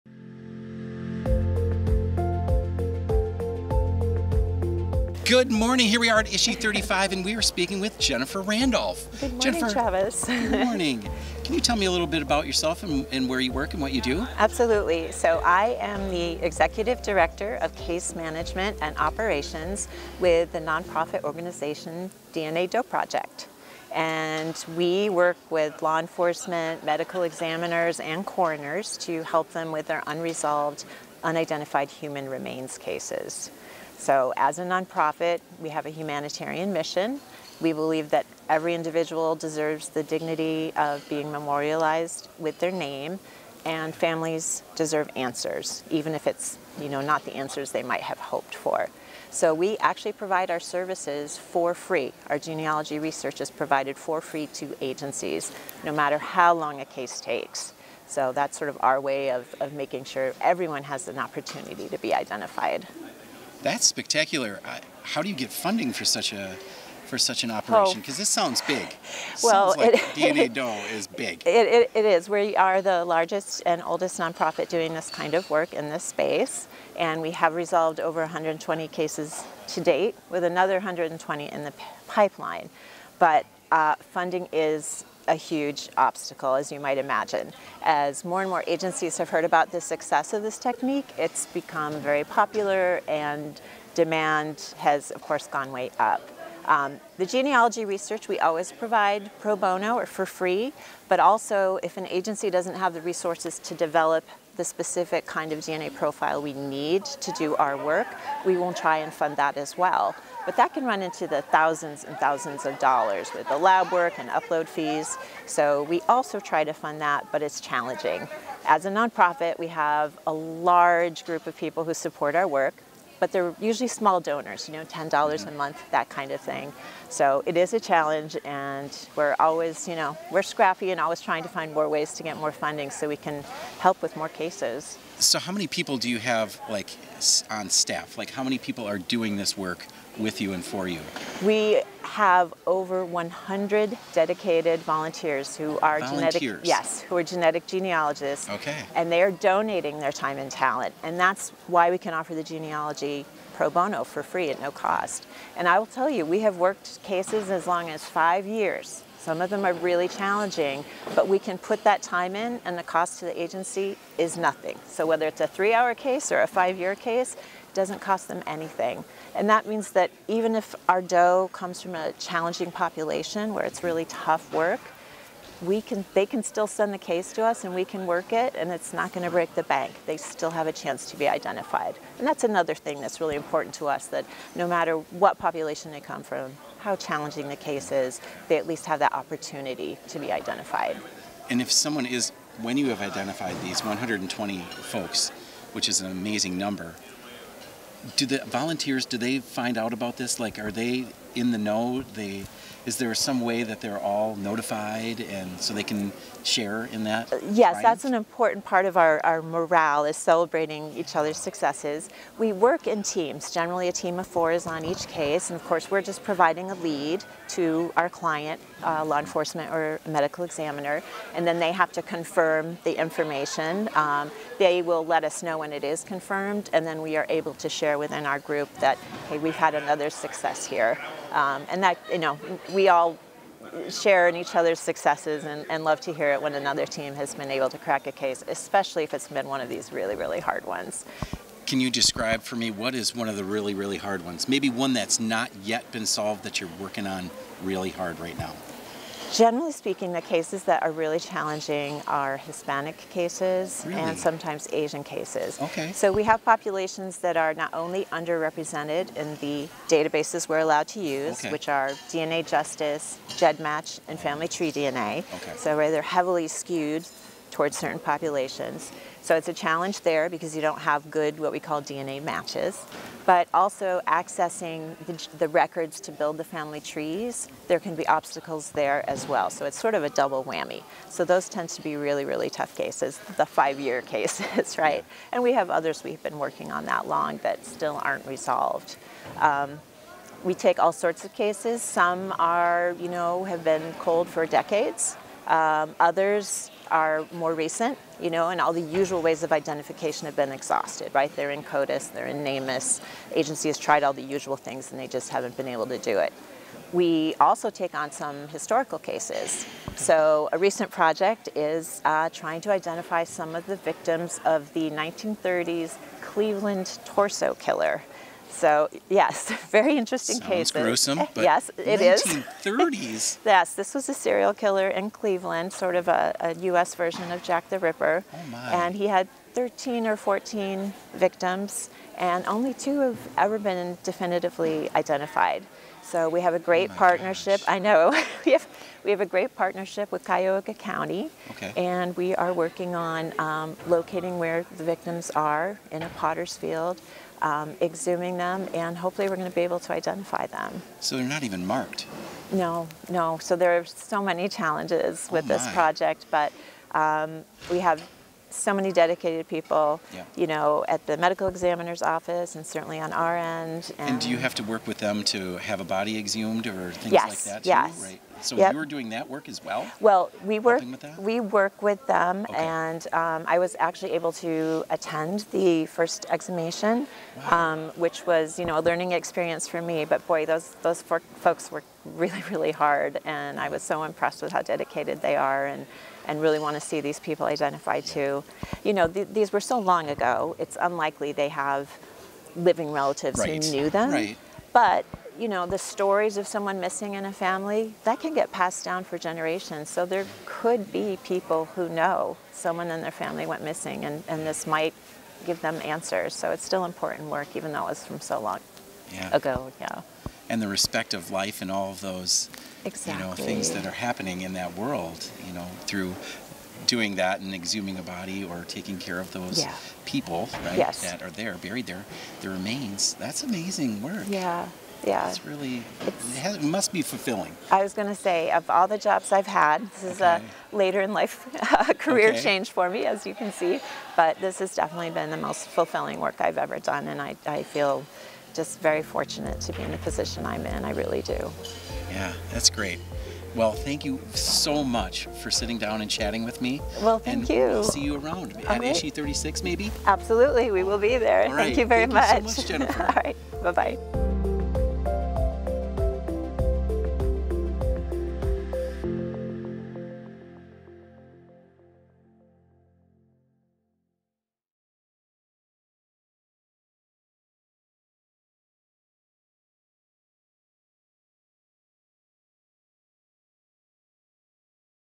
From Nameless to Known: How DNA Doe Project Brings Justice to the Forgotten
Imagine working for years to provide answers to just one family. At ISHI 35, we sat down with Jennifer Randolph, Executive Director of Case Management at DNA Doe Project, to discuss how this nonprofit is transforming forensic genealogy and giving names back to the unknown. With over 100 dedicated volunteer genealogists, DNA Doe Project has tackled some of the most challenging cases, from recent unidentified remains to victims of the 1930s Cleveland Torso Killer.

🌍 Learn about DNA Doe Project’s humanitarian mission and why they’re committed to providing DNA research services for free, regardless of how challenging the case may be.

🔬 Learn how a dedicated team of over 100 volunteer genealogists tackles even the toughest cases, from unidentified remains in recent cases to forgotten victims of the 1930s Cleveland Torso Killer.

💡 Explore the real challenges and breakthroughs in forensic genealogy, from limited funding and population representation to complex cases that require years of patient, painstaking research.

Discover how DNA Doe Project is reshaping cold case investigations, and bringing dignity to the lives of those who had been left nameless.